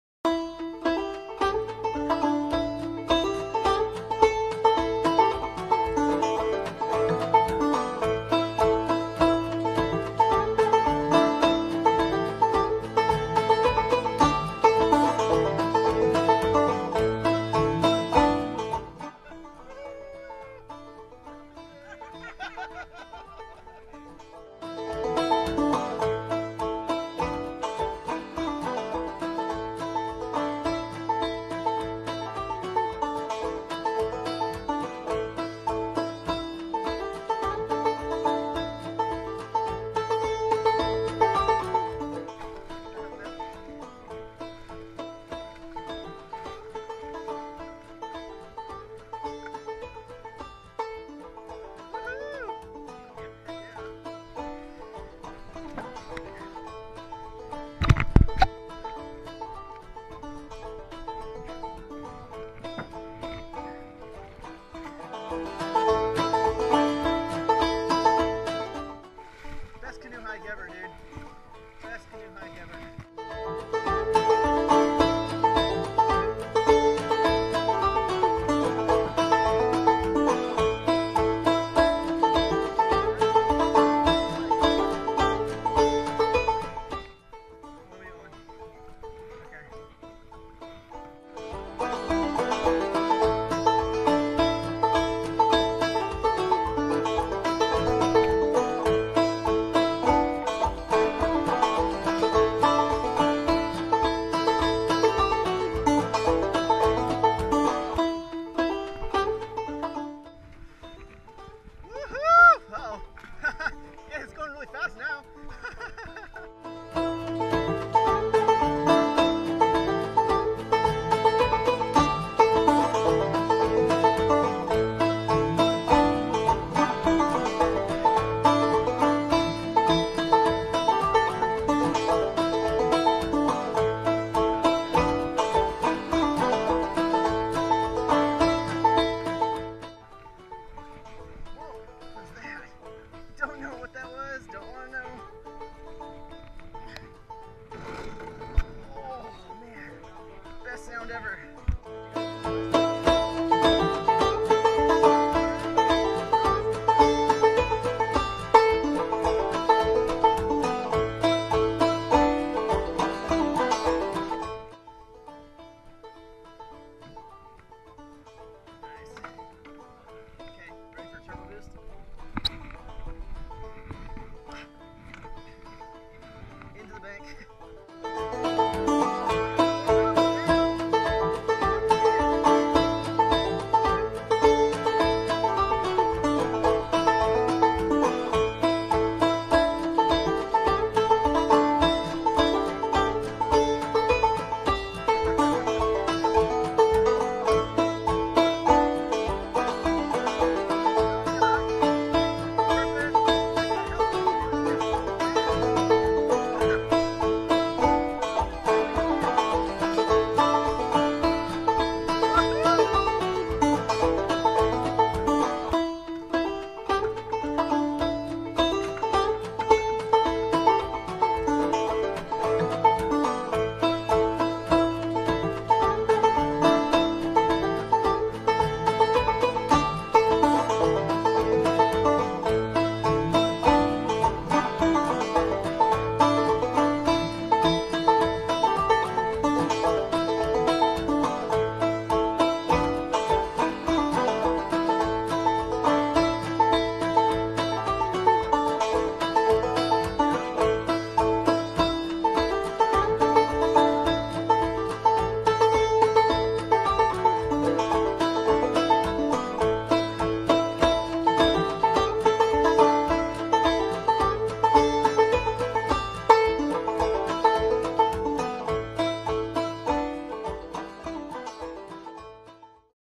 Wild Hog Canoe Race 2017
Peter and I take on the Wild Hog Canoe Race for the second time. What a wild ride.